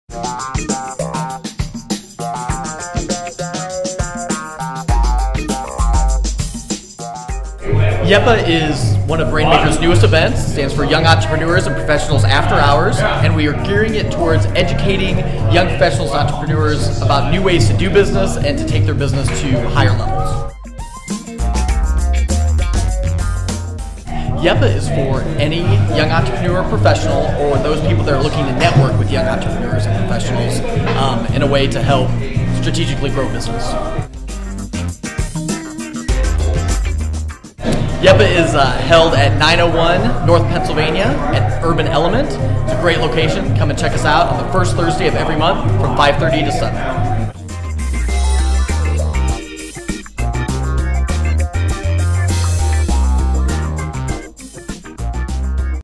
Rainmakers YEPA
Rainmakers Young Entrepreneurs and Professionals After Hours explains why you need to be attending this event!